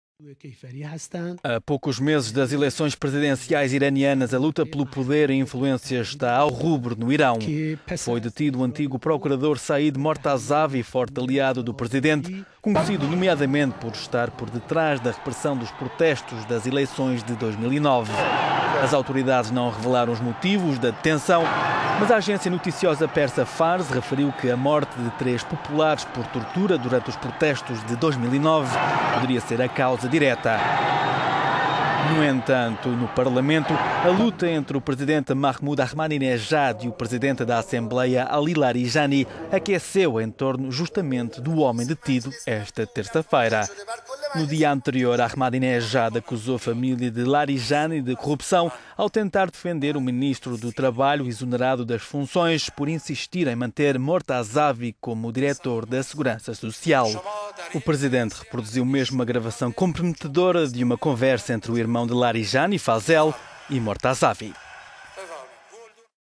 Detenção e lavagem de roupa suja em guerra pelo poder
A poucos meses das eleições presidenciais iranianas a luta pelo poder e influência está ao rubro no Irão.

Foi detido o antigo procurador Saeed Mortazavi, forte aliado do presidente, conhecido entre coisas por estar por detrás da repressão aos protestos das eleições de 2009.

As autoridades não revelaram os motivos da detenção, mas a agência noticiosa persa FARS referiu que a morte de três populares por tortura durante os protestos de 2009 poderia ser a causa direta. Um caso que provocou a suspensão do cargo na justiça.

No entanto, no Parlamento, a luta entre o presidente, Mahmoud Ahmadinejad, e o presidente da Assembleia, Ali Larijani, aqueceu em torno justamente do homem detido esta terça-feira.

No dia anterior, Ahjmanidenjad acusou a família de Larijani de corrupção ao tentar defender o ministro do Trabalho, exonerado das funções por insistir em manter Mortazavi como diretor da Segurança Social.

O presidente reproduziu mesmo uma gravação comprometedora de uma conversa entre o irmão de Larijani, Fazel, e Mortazavi.